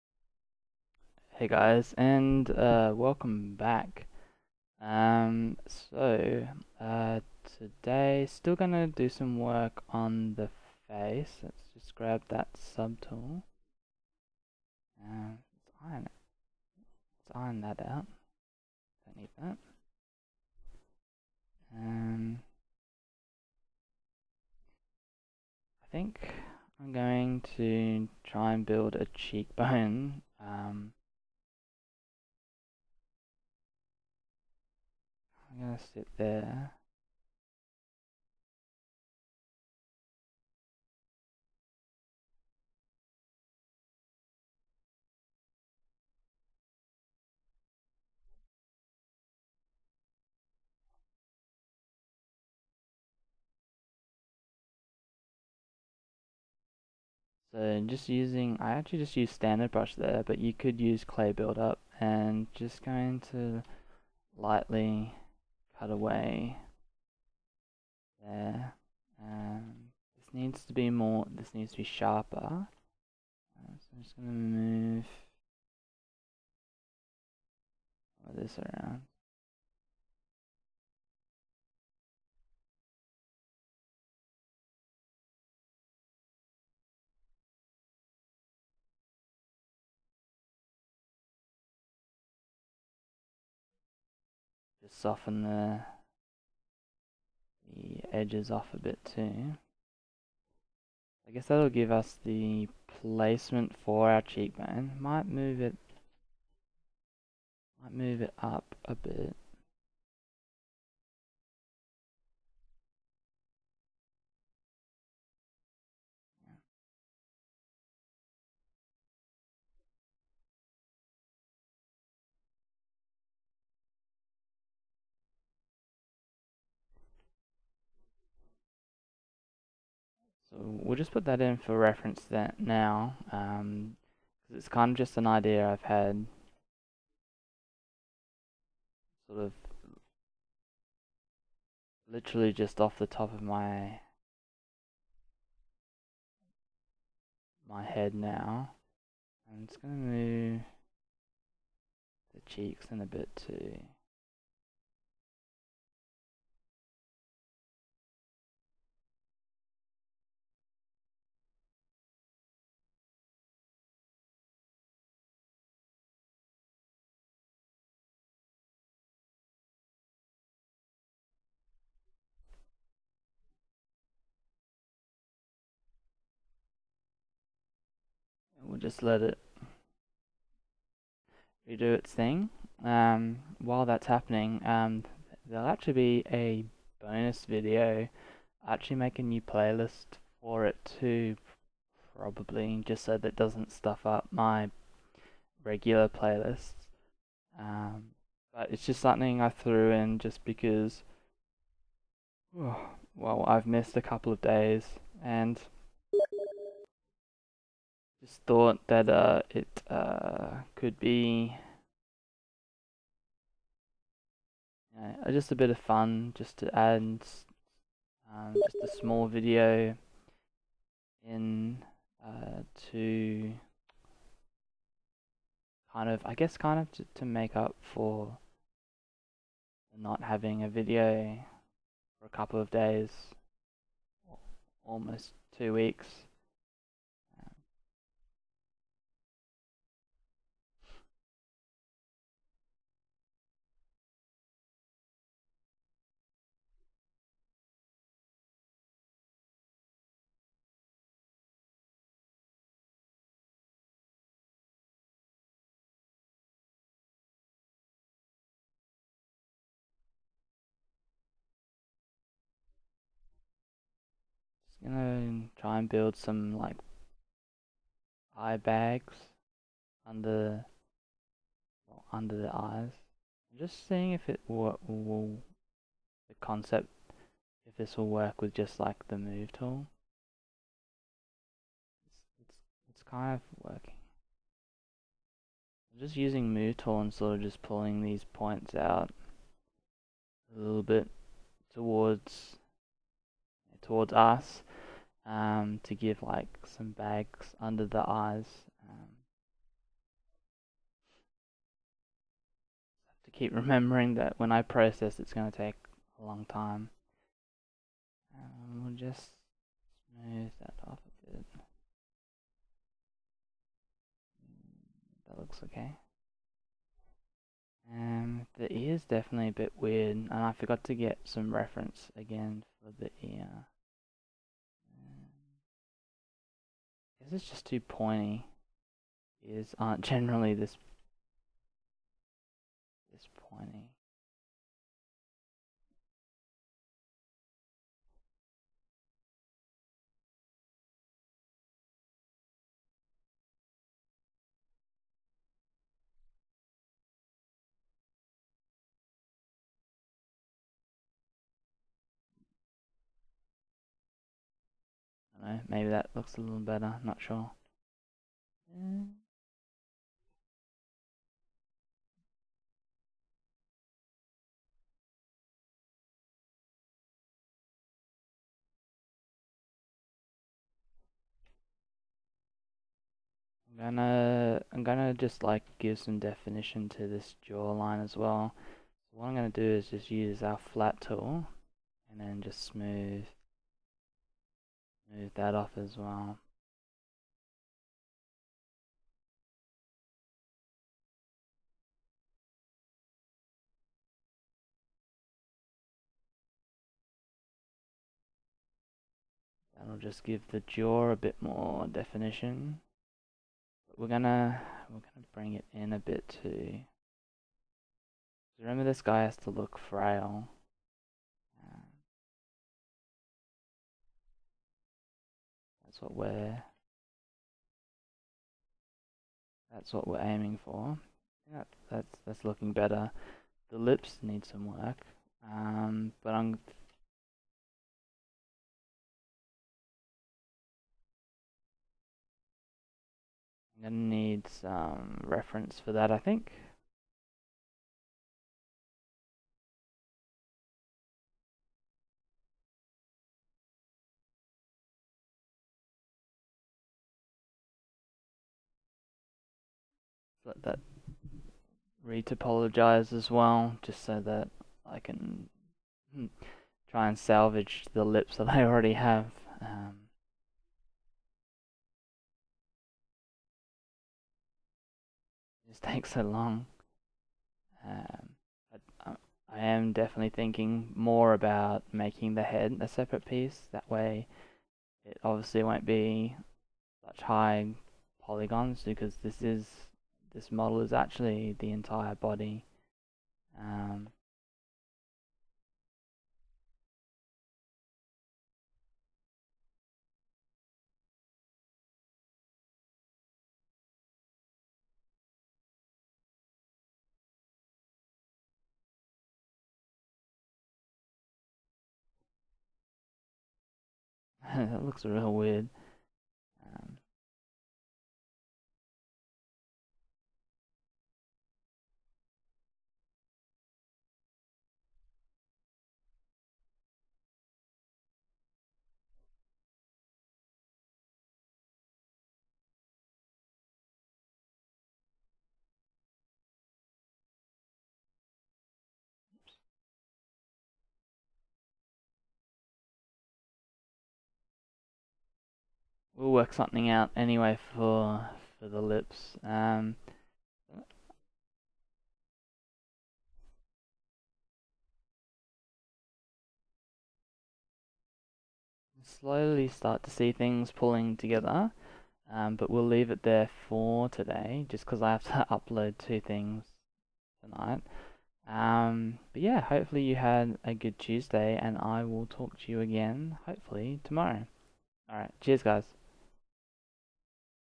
Meshing About Vlog - Cheeky
Adding in some cheek and cheek bones to the fragile face.